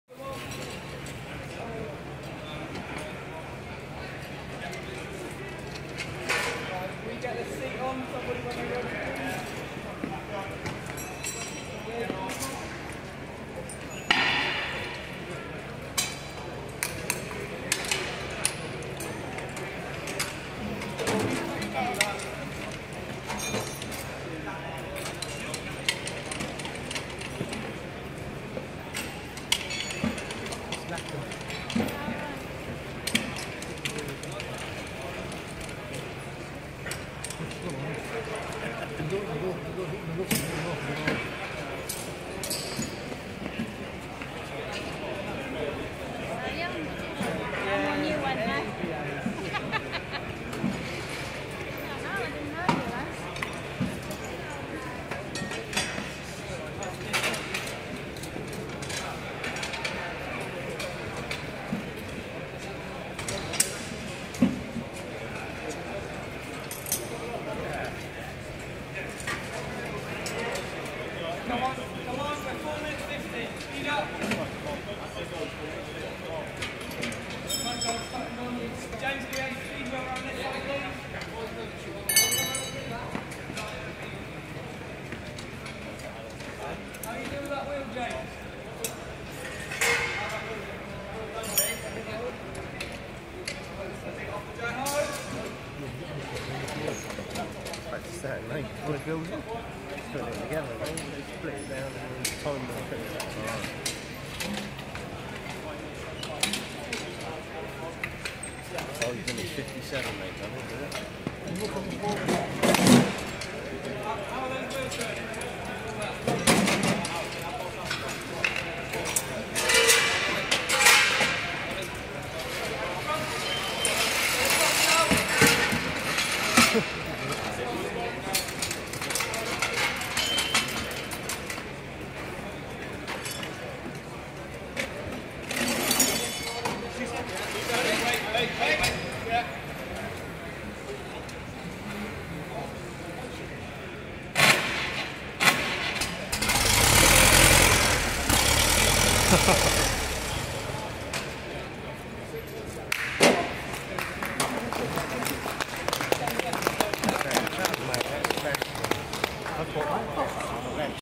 Skladanie traktora
Poskladanie traktora za menej ako 6min. aj z nastartovanim...